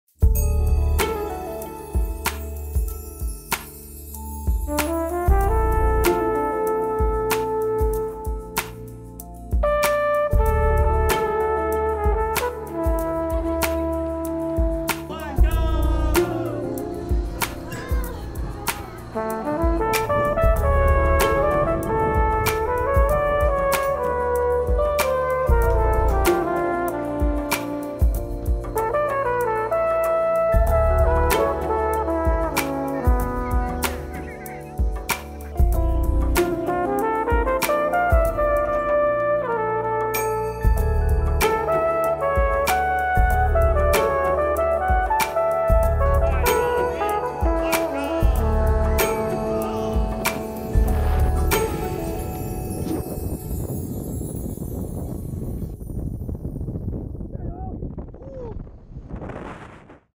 Bungy jumping from world's highest glass-bottomed bridge
This glass-bottomed suspension bridge in China offers bungy jumping to the brave. Would you bungy jump from the world’s highest glass-bottomed? See what it's like.